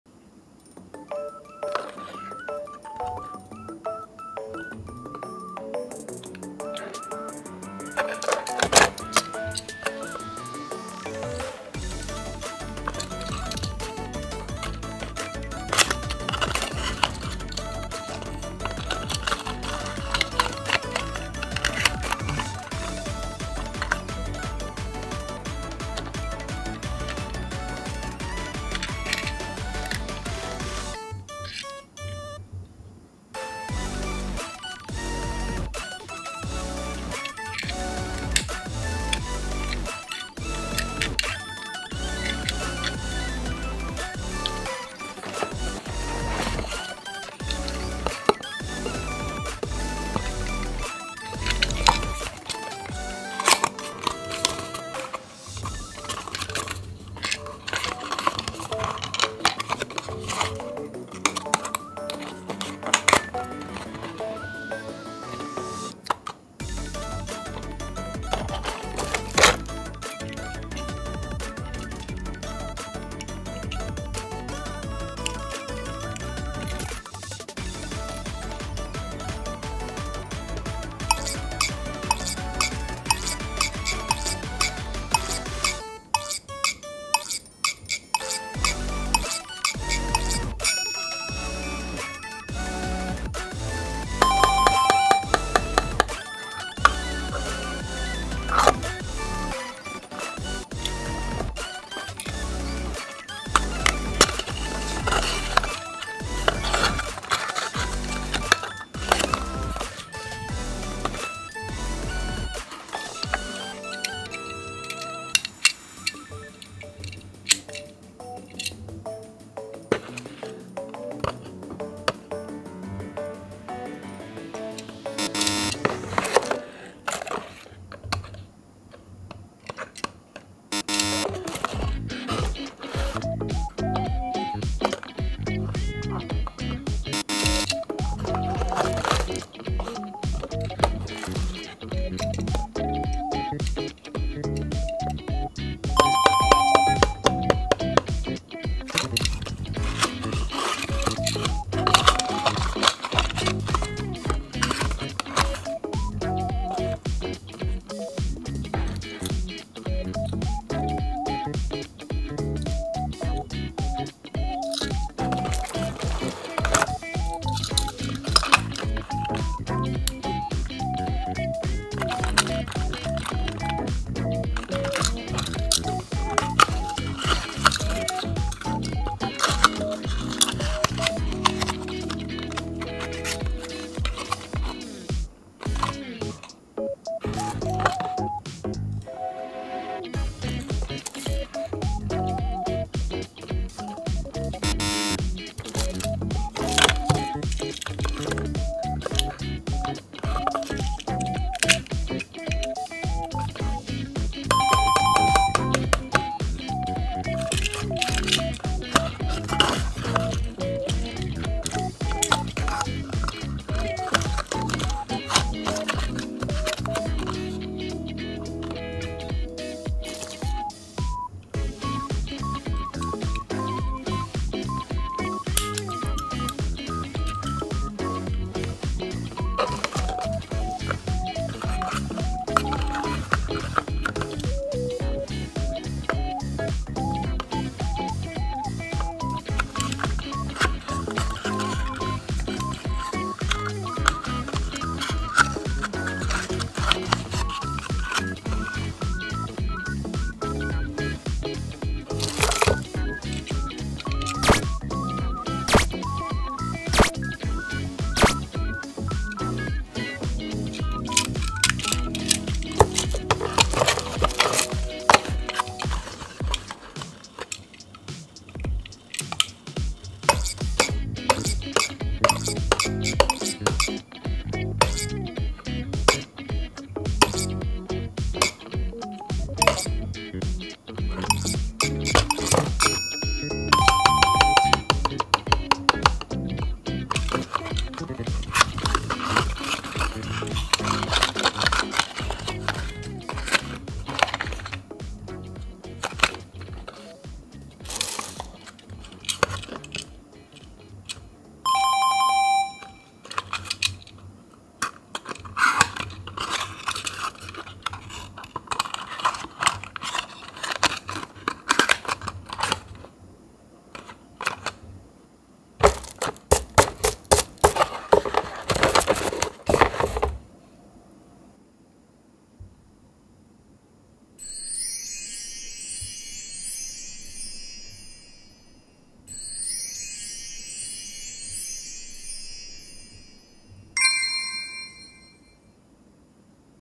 トミカ『救急車』ミニカーと同じ絵柄の箱を探し収納しよう！緊急車両 Look for boxes with the same design as the ambulance and store them
はたらくくるまの中で人気の「救急車（緊急車両）」のミニカーを選びました。１台ずつ箱に戻します。
We choose a mini car of the popular "Ambulance (Emergency Vehicle)" among working cars. Return them to the box one by one.